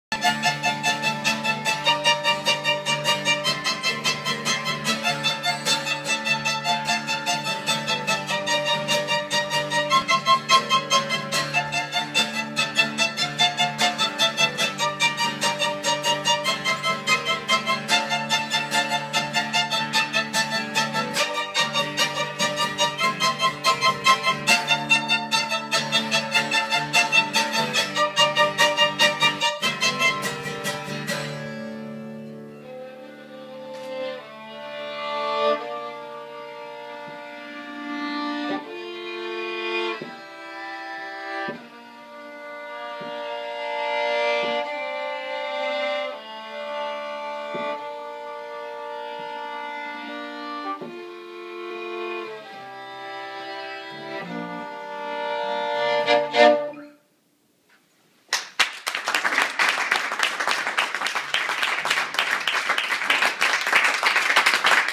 Jason Howell and Ben Deaton
A short clip of Jason and Ben getting down in some stringed instruments.